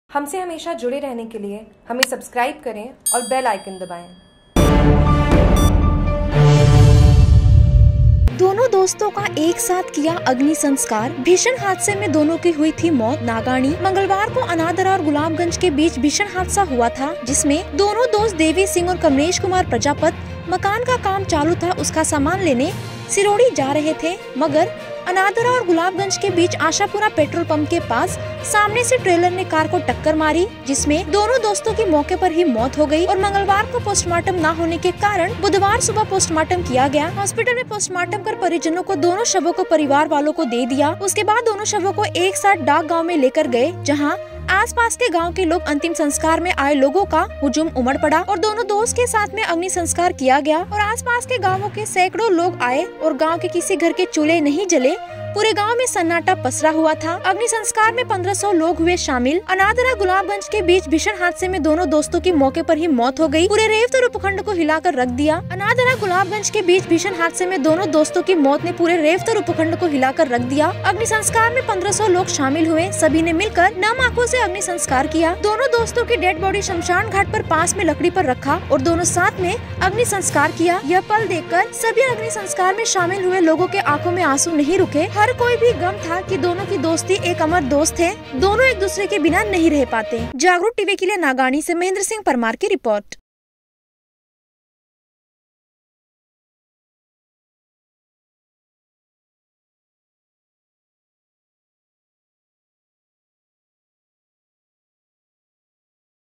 नागाणी : दोनों दोस्तों को एक साथ किया अग्नि संस्कार, भीषण हादसे में दोनों की हुईं थी मौत
मंगलवार को अनादरा और गुलाबगंज के बींच भीषण हादसा हुआ था जिसमें दोनों दोस्त देवी सिंह और कमलेश कुमार प्रजापत मकान का काम चालू था उसका सामान लेने सिरोडी जा रहें थे मगर अनादरा और गुलाबगंज के बींच आशापुरा पेट्रोल पंप के पास सामने से टेलर ने कार को टक्कर मारी जिसमें दोनों दोस्तों की मौके पर मौत हुई और मंगलवार को पोस्टमार्टम ना होने के कारण बुधवार सुबह पोस्टमार्टम किया गया हॉस्पिटल में पोस्टमार्टम कर परिजनों को दोनों शवों को परिवार वालों दिया उसके बाद दोनों शवों को एक साथ डाक गांव में लेकर गए जहां आसपास के गांव के लोग अंतिम संस्कार में आय लोगों का हुजूम उमड़ पड़ा और दोनों दोस्तों के साथ में अग्नि संस्कार किया गया और आसपास गांवो से सैकड़ो लोग आये और गांव किसी के घर चूले नही जले पूरे गांव में सन्नाटा पसरा हुआ था

अग्नि संस्कार में 1500 लोग हुए शामिल

अनादरा गुलाबगंज के बीच भीषण हादसे में दोनों दोस्तों की मौत ने पूरे रेवदर उपखंड को हिलाकर रख दिया अग्नि संस्कार में 1500 लोग शामिल हुऐ सभी ने मिलकर नम आंखो से अग्नि संस्कार किया

दोनों दोस्तों की डेड बॉडी श्मशान घाट पर पास में लकड़ी पर रखा और दोनों साथ में अग्नि संस्कार किया यह पल देखकर सभी  अग्नि संस्कार में शामिल हुए लोगो के आंखों में आंसू नहीं रुके हर कोई भी गम था कि दोनों की दोस्ती एक अमर दोस्त थे दोनों एक दूसरे के बिना नहीं रह पाते